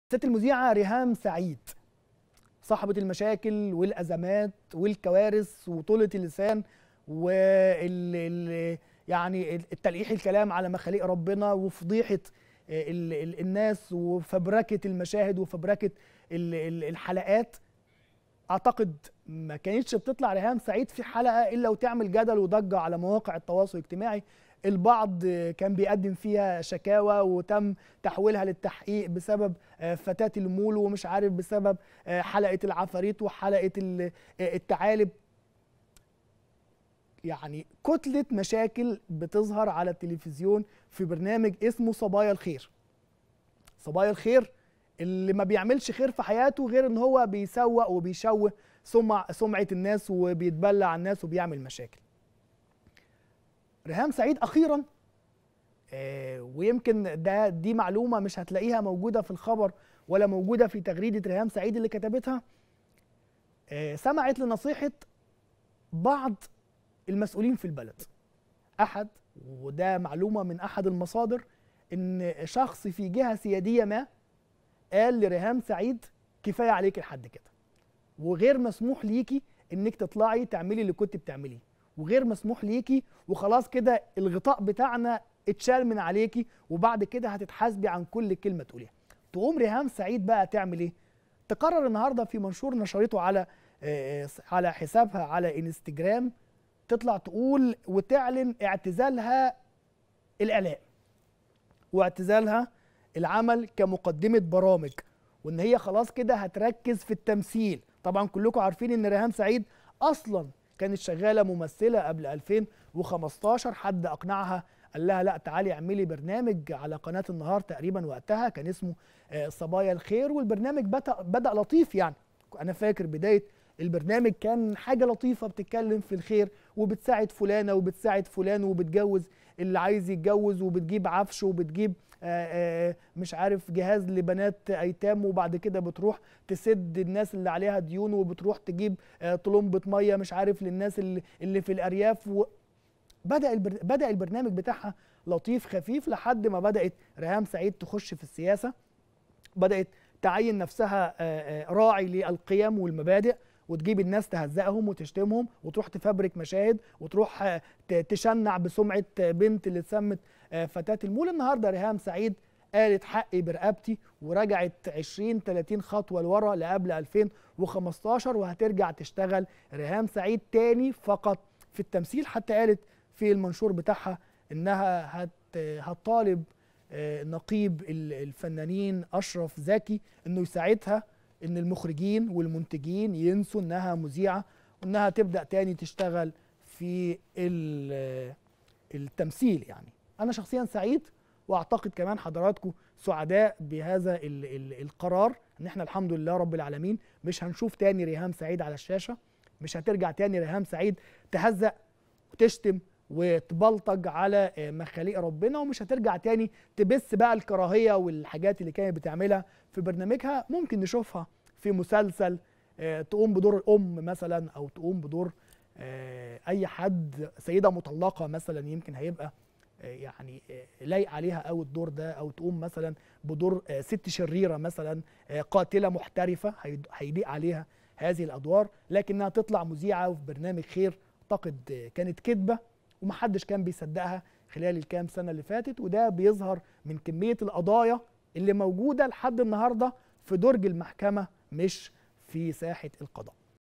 ريهام سعيد تعتزل بأمر من جهة سيادية
ريهام سعيد تعتزل الإعلام بأمر من جهة سيادية